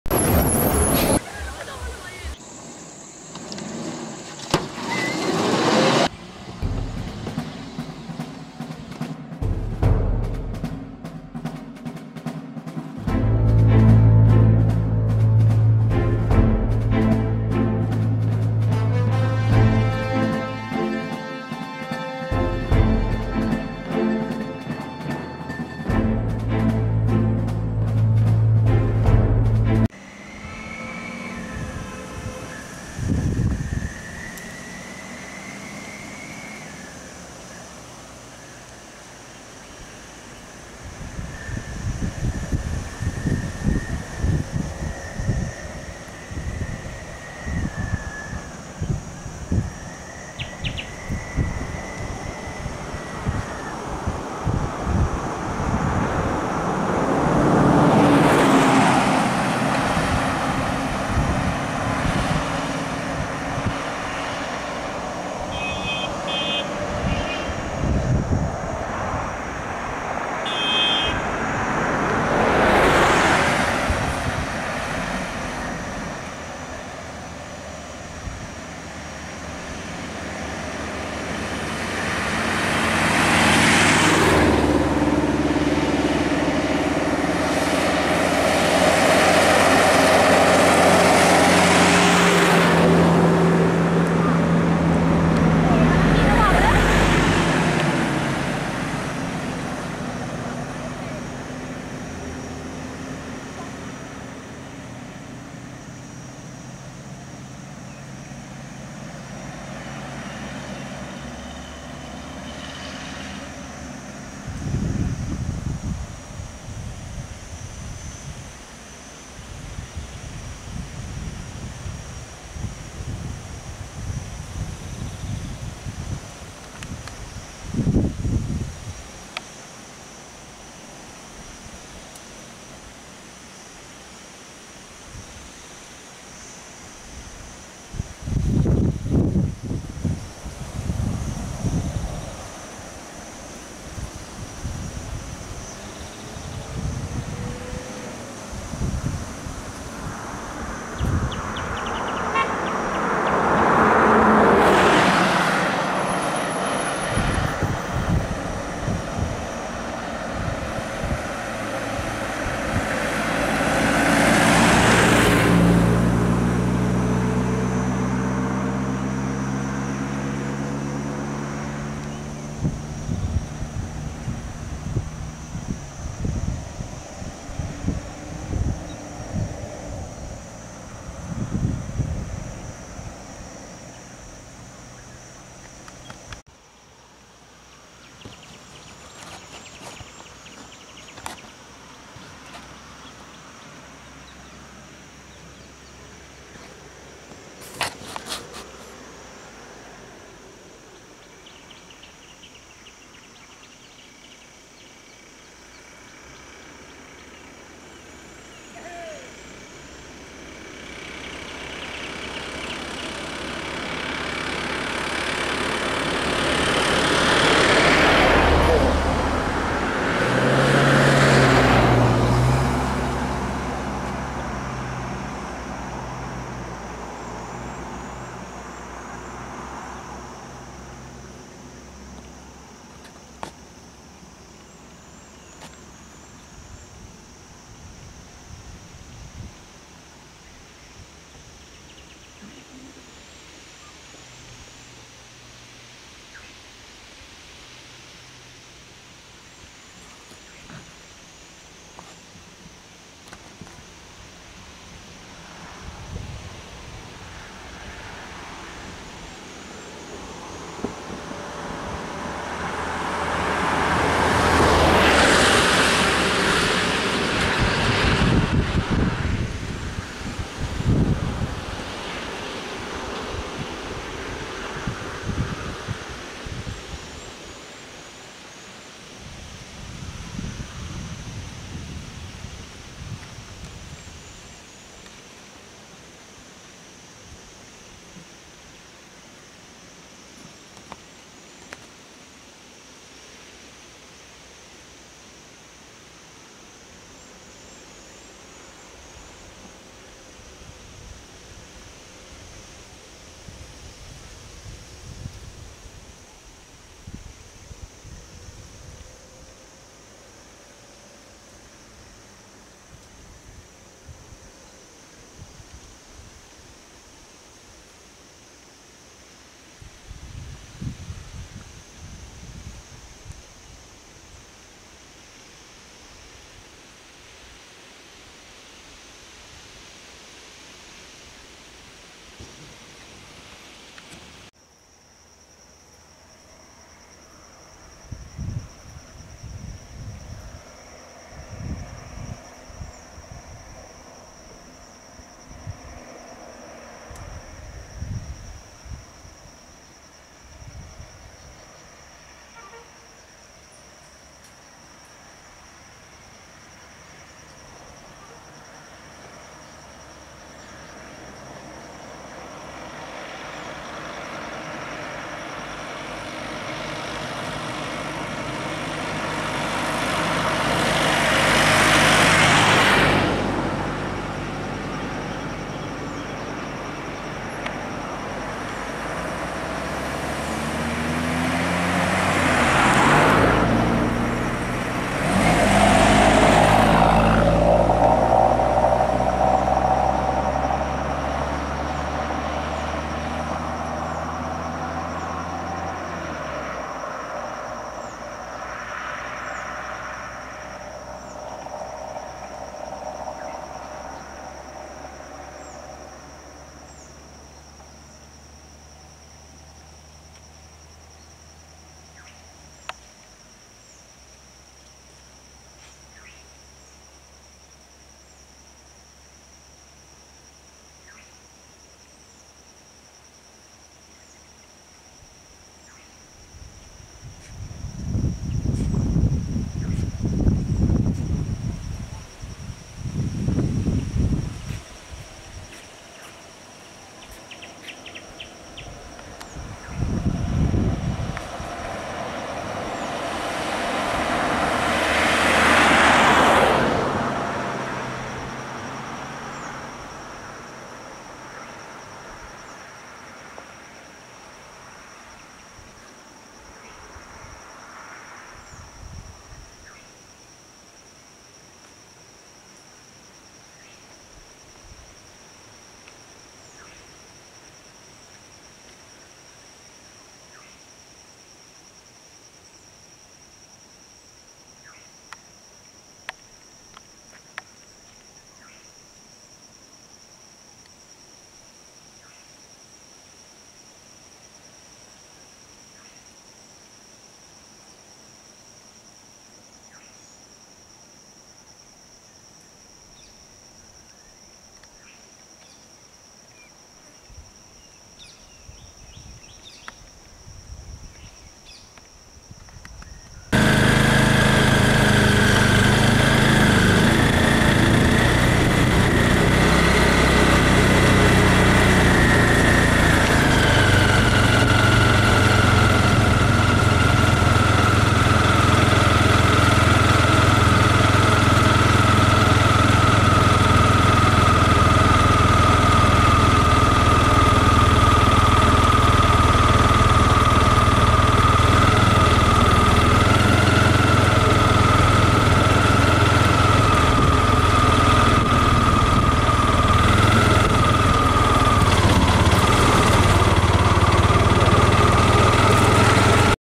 aggressive tusker on the road
so many time i tried to catch him on my camera i was lucky this time i was able to catch him small man but little bit aggressive on the road may be he is not used to road much more